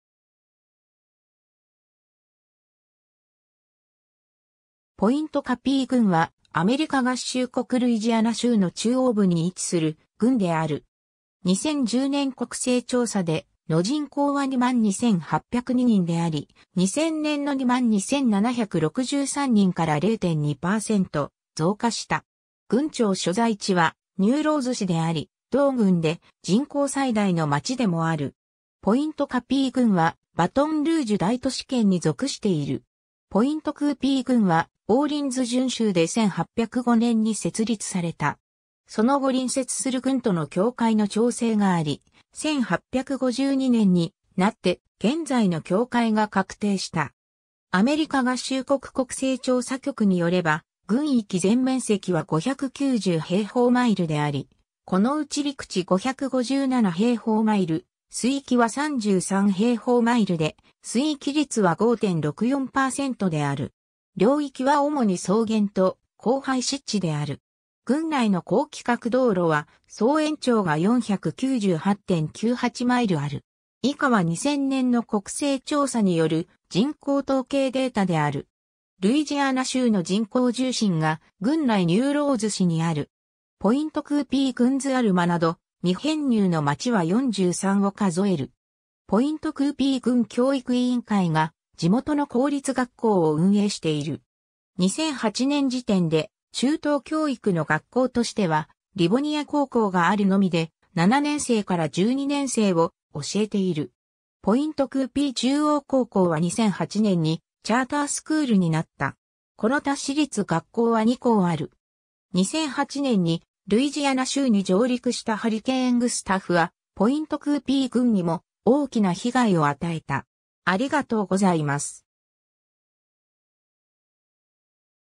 ポイントクーピー郡 (ルイジアナ州)
ポイントカピー郡（英: Pointe Coupee Parish、英語の発音は "Point Coo-Pea"）は、アメリカ合衆国ルイジアナ州の中央部に位置する郡である。
2010年国勢調査での人口は22,802人であり、2000年の22,763人から0.2％増加した。
郡庁所在地はニューローズ市（人口4,831人）であり、同郡で人口最大の町でもある。
 ポイントカピー郡はバトンルージュ大都市圏に属している。
 ポイントクーピー郡はオーリンズ準州（1812年にルイジアナ州になった）で1805年に設立された。
その後隣接する郡との境界の調整があり、1852年になって現在の境界が確定した。
領域は主に草原と後背湿地である。
 郡内の高規格道路は総延長が498.98マイル (798 km) ある。
 ルイジアナ州の人口重心が郡内ニューローズ市にある。
 ポイントクーピー郡図 アルマなど未編入の町は43を数える。
 ポイントクーピー郡教育委員会が地元の公立学校を運営している。
 2008年時点で中等教育の学校としてはリボニア高校があるのみで、7年生から12年生を教えている。
ポイントクーピー中央高校は2008年にチャーター･スクールになった。
この他私立学校は2校ある。
 2008年にルイジアナ州に上陸したハリケーン・グスタフはポイントクーピー郡にも大きな被害を与えた。